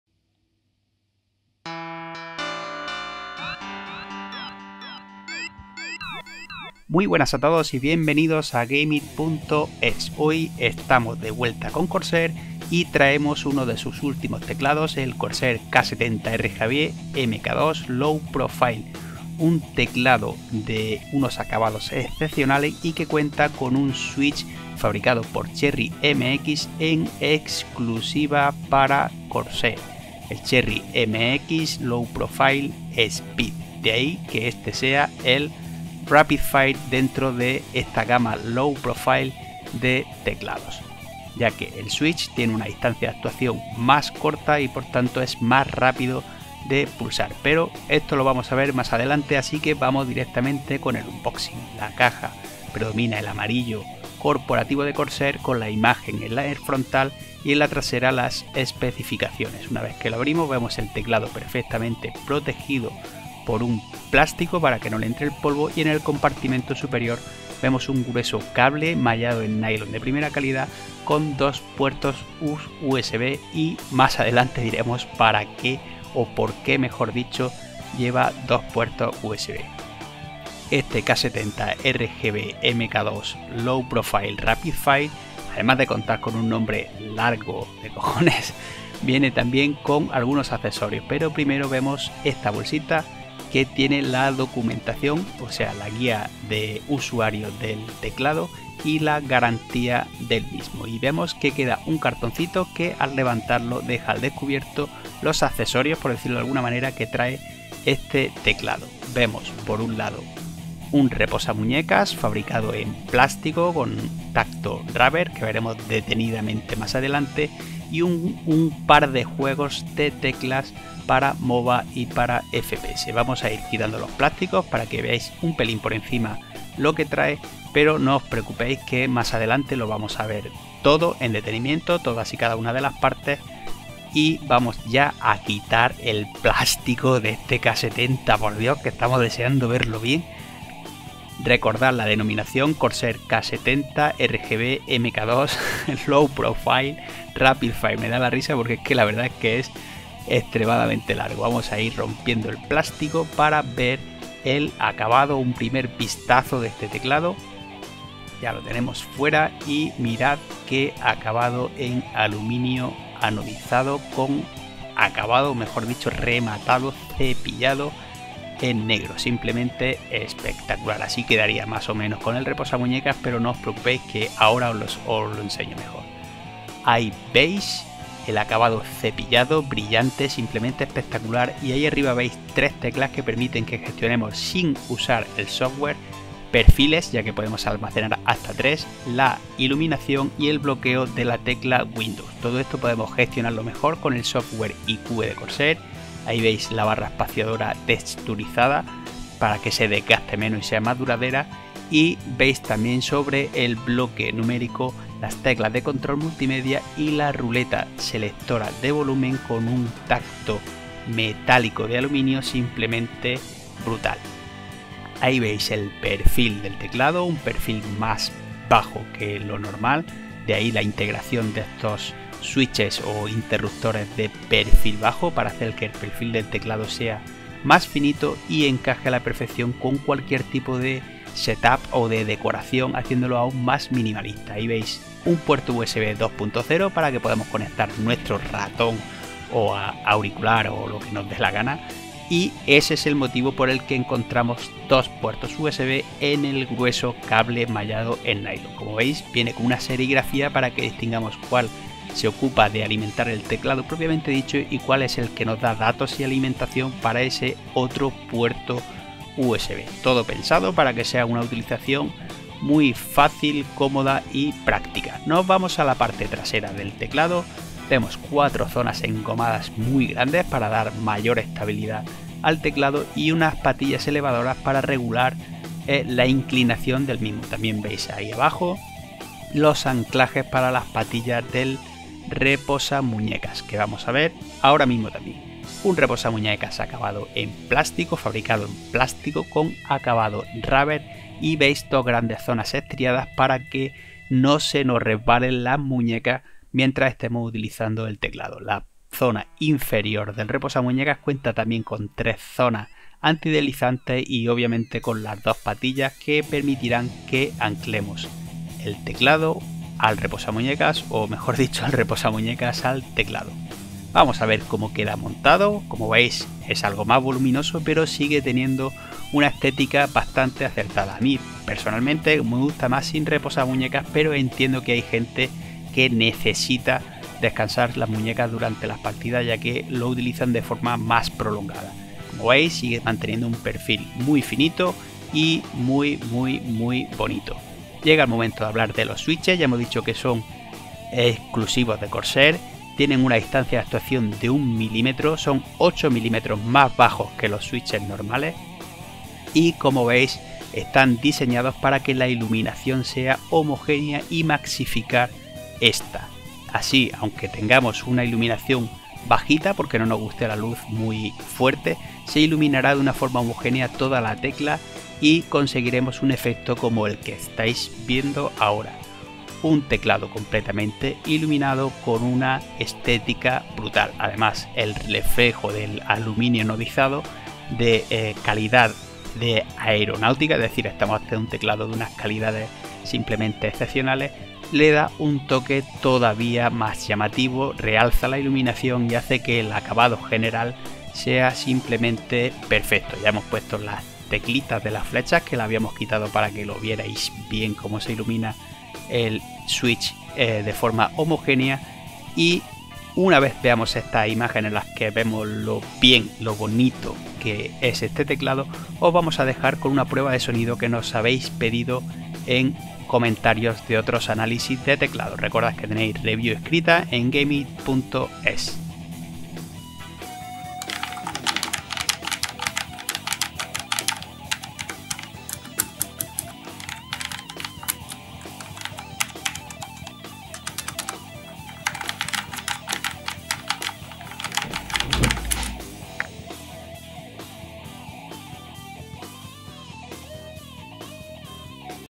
Corsair K70 RGB MK2 Low Profile Rapidfire, unboxing en español
Unboxing y review en español del teclado mecánico Corsair K70 RGB MK2 Low Profile Rapidfire, un teclado con cherry mx speed low profile, marco en aluminio anodizado de calidad aeronáutica y una estética espectacular